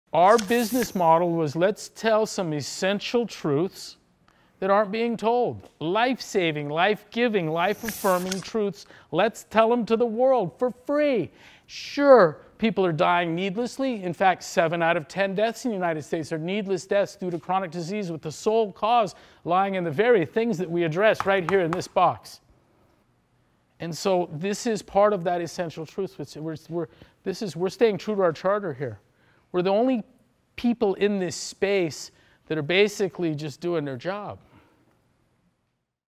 Doing Our Job
"We're doing our job," says CrossFit Founder Greg Glassman as he talks to a crowd at CrossFit New England.